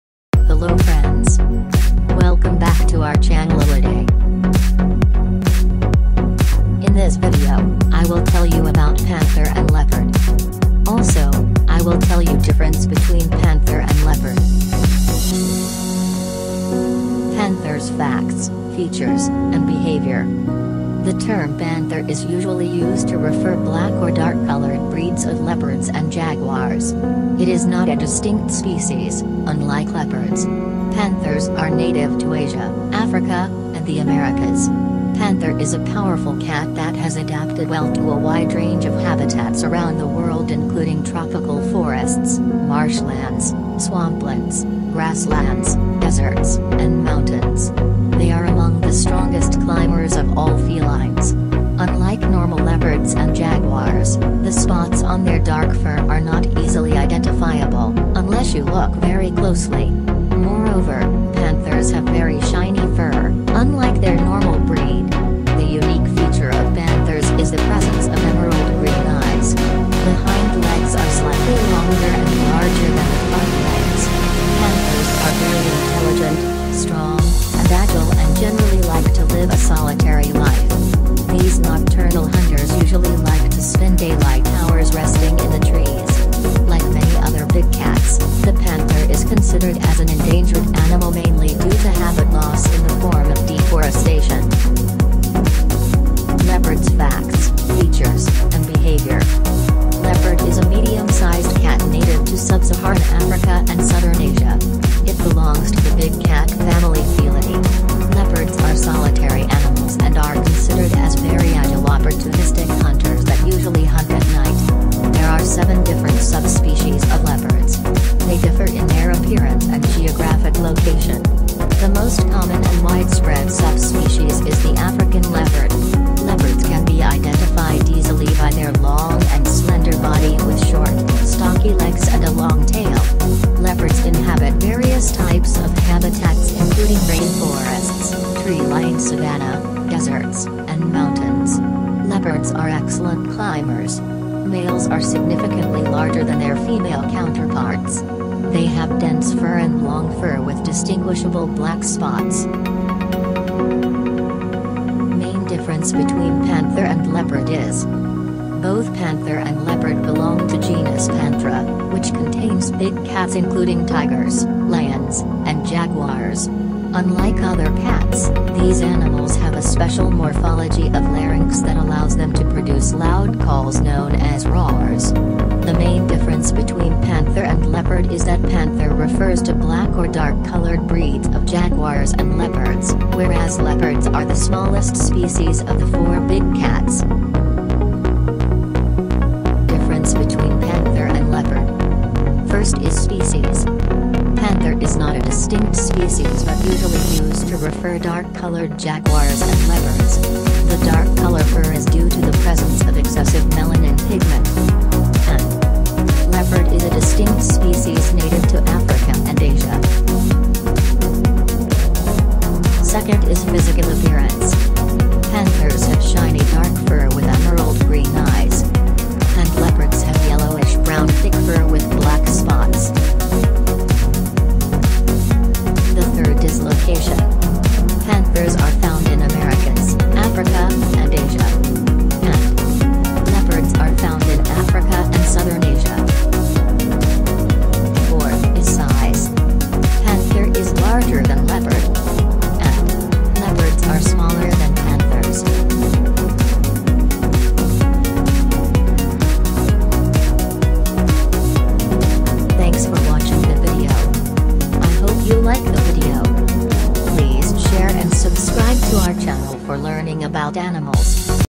Difference between panther and leopard | Panther vs Leopard
what is the Difference between Panther and Leopard?

Main Difference -Panther vs Leopard
Both panther and leopard belong to the genus Panthera, which contains big cats including tigers, lions, and jaguars. Unlike other cats, these animals have a special morphology of larynx that allows them to produce loud calls known as roars. The main difference between panther and leopard is that panther refers to black or dark colored breeds of jaguars and leopards, whereas leopards are the smallest species of the four big cats.

Panther: Facts, Features, and Behaviour.
The term panther is usually used to refer to black or dark-colored breeds of leopards and jaguars. It is not a distinct species, unlike leopards. Panthers are native to Asia, Africa, and the Americas. Panther is a powerful cat that has adapted well to a wide range of habitats around the world including tropical forests, marshlands, swamplands, grasslands, deserts, and mountains. They are among the strongest climbers of all felines. Unlike normal leopards and jaguars, the spots on their dark fur are not easily identifiable, unless you look very closely. Moreover, panthers have very shiny fur, unlike their normal breed. The unique feature of panthers is the presence of emerald green eyes. The hind legs are slightly longer and larger than the front legs. Panthers are very intelligent, strong, and agile and generally like to live a solitary life. These nocturnal hunters usually like to spend daylight hours resting in the trees. Like many other big cats, the panther is considered as an endangered animal mainly due to habit loss in the form of deforestation.

Leopard – Facts, Features, and Behaviour
Leopard is a medium-sized cat native to sub-Saharan Africa and southern Asia. It belongs to the big cat family Felidae. Leopards are solitary animals and are considered as very agile opportunistic hunters that usually hunt at night. There are seven different subspecies of leopards. They differ in their appearance and geographic location. The most common and widespread subspecies is the African leopard. Leopards can be identified easily by their long and slender body with short, stocky legs and a long tail. Leopards inhabit various types of habitats including rainforests, tree-lined savannah, deserts, and mountains. Leopards are excellent climbers. Males are significantly larger than their female counterparts. They have dense fur and long fur with distinguishable black spots.

Difference between Panther and Leopard.
1.Species.
Panther is not a distinct species but usually used to refer dark-colored jaguars and leopards. The dark color fur is due to the presence of excessive melanin pigment.
Leopard is a distinct species native to Africa and Asia.

2.Physical Appearance.
Panthers have shiny dark fur with emerald green eyes.
And  Leopards have yellowish-brown thick fur with black spots.

3. Location
Panthers are found in the Americas, Africa, and Asia
Leopards are found in Africa and southern Asia.

4. Size.
The panther is larger than the leopard.
And. Leopards are smaller than panthers.